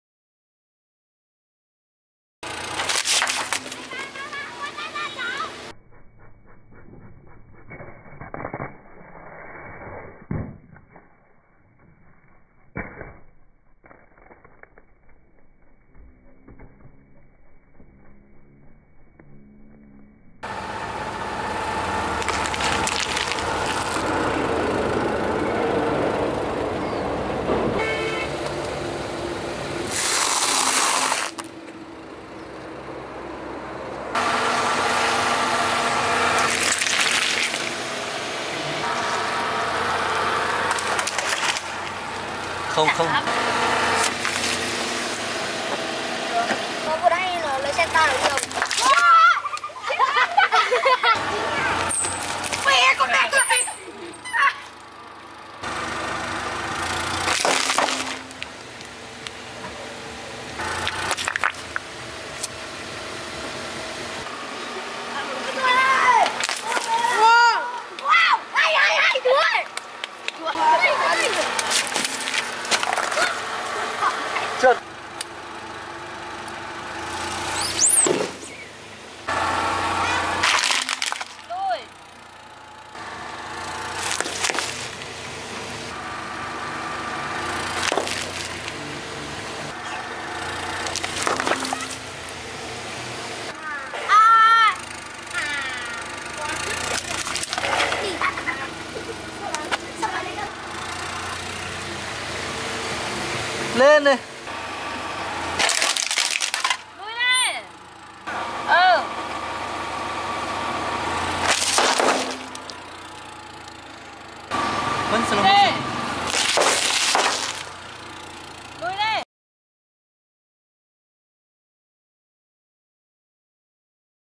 Experiment Car vs Toothpaste | Crushing Crunchy & Soft Things by Car!
WARNING! Dangerous Crash Test, don´t Try this by yourself!! Experiment Car vs Toothpaste.